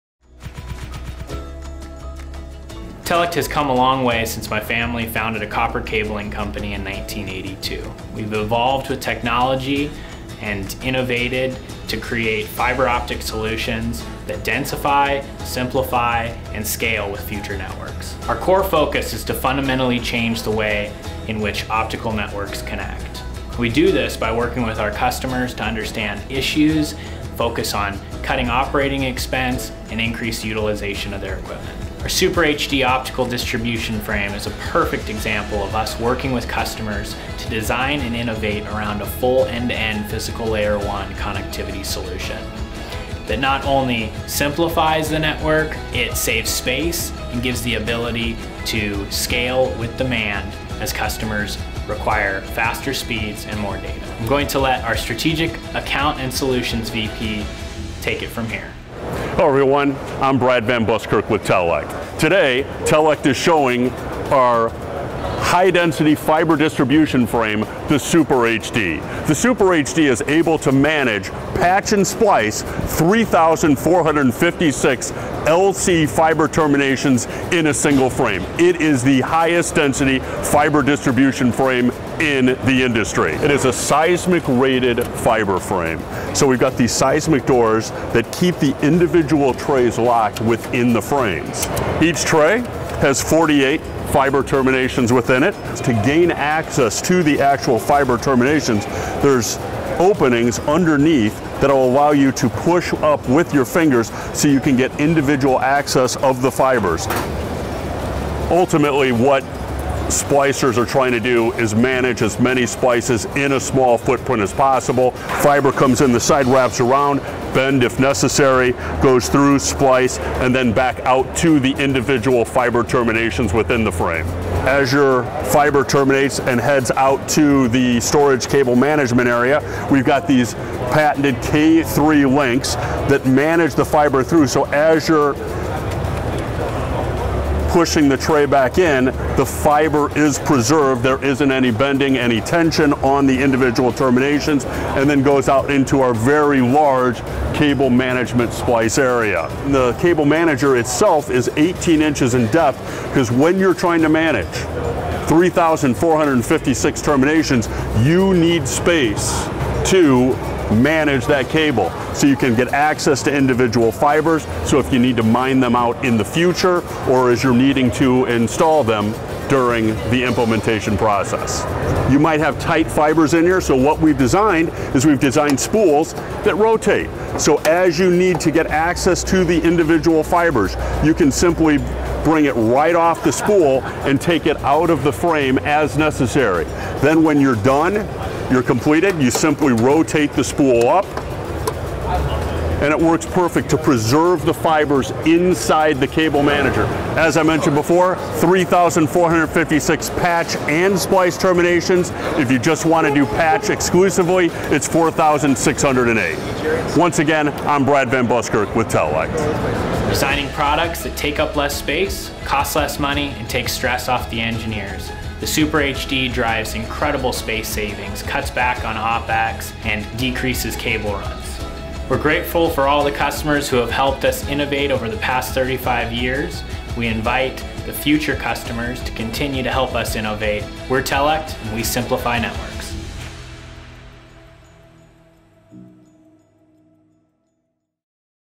The Future of Telect and Fiber Optic Technology
Spencer Williams, the Vice President of Sales, Marketing and Product Strategy for Telect, talks about the future for the company, focusing its efforts on fiber optic technology and simple Layer 1 technology, customized for each customer.

Our superHD Fiber Distribution Frame is a perfect example of Telect collaborating with our clients to design Layer 1 solutions to simplify networks and reduce costs.